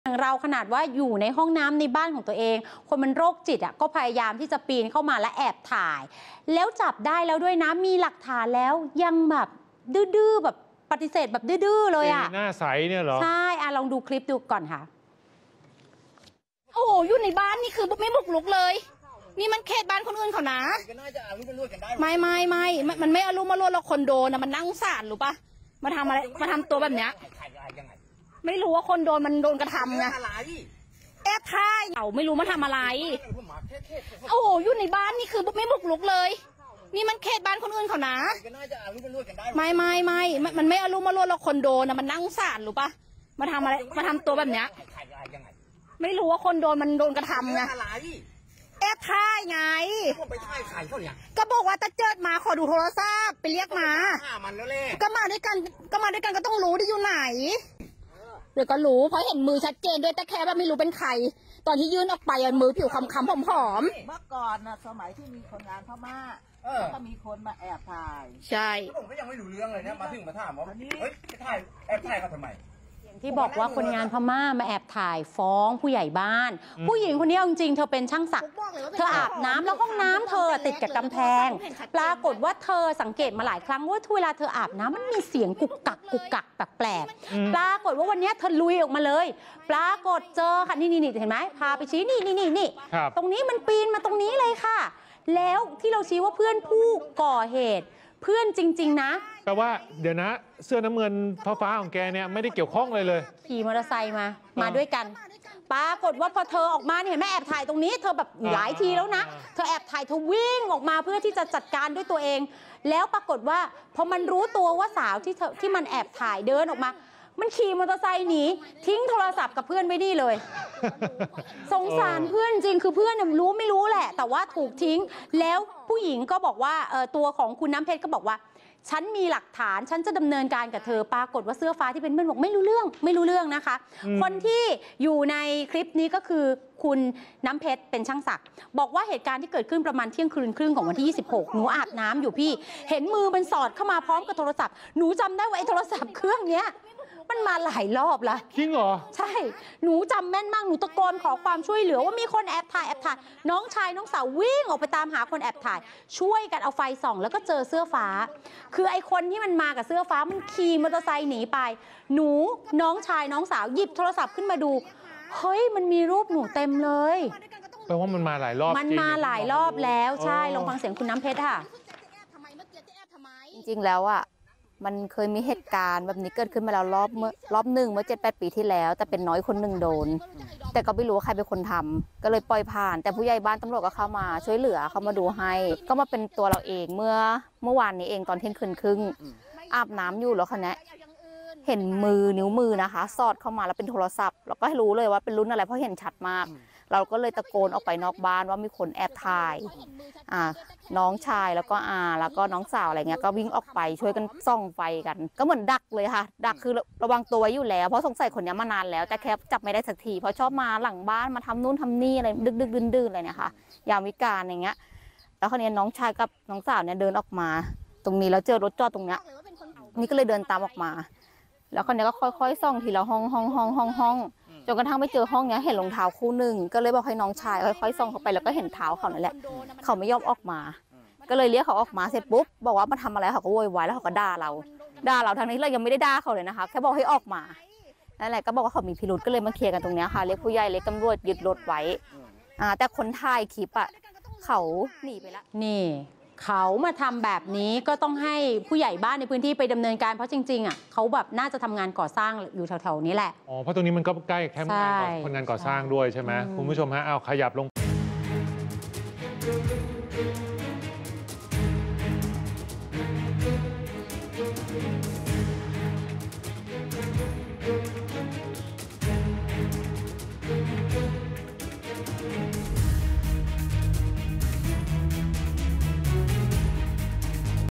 สาวช่างสักถูกโรคจิตปีนบ้านแอบถ่ายขณะอาบน้ำ ผงะเจอคลิปตัวเองในมือถือเพียบ | ทุบโต๊ะข่าว | 27/05/66
สาวช่างสักถูกโรคจิตปีนบ้านแอบถ่ายขณะอาบน้ำ ผงะเจอคลิปตัวเองในมือถือเพียบ